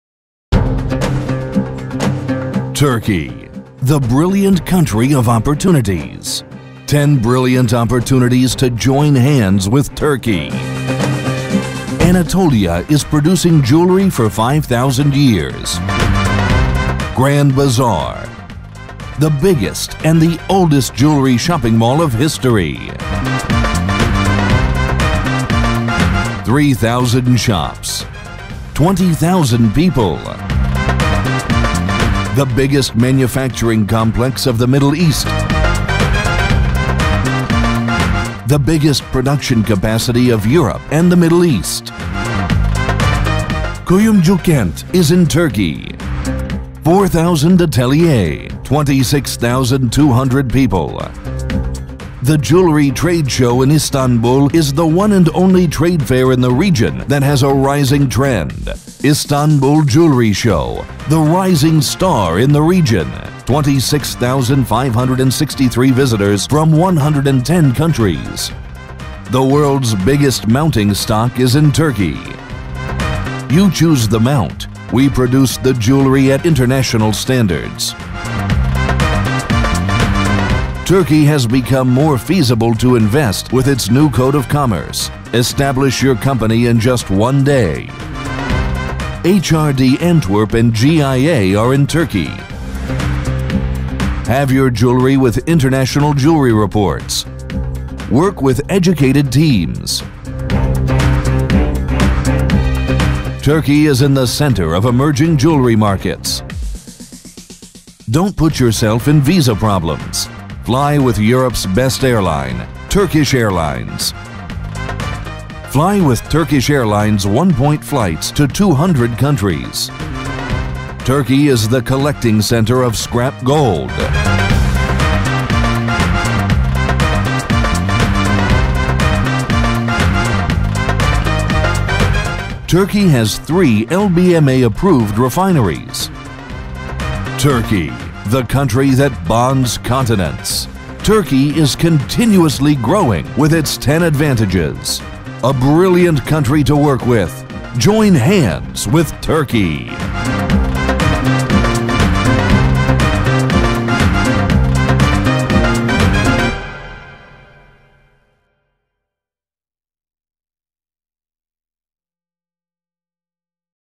Mücevher İhracatçıları Birliği (JTR) - Türkiye'nin Fırsatları
3K1A: Music Composition & Production, Dubbing

Brand: Mücevher İhracatçıları Birliği (JTR)
Campaign: Türkiye'nin Fırsatları (10 Brilliant Oppurtunities Of Turkey)
Media: Meetings, Fairs
3K1A Credits
Producer: Onur Öztürk, Seyran Kurtuluş Öztürk
Composing, Recording, Mixing, Mastering: Gürkan Bozacı
Arrangement: Gürkan Bozacı, Onur Öztürk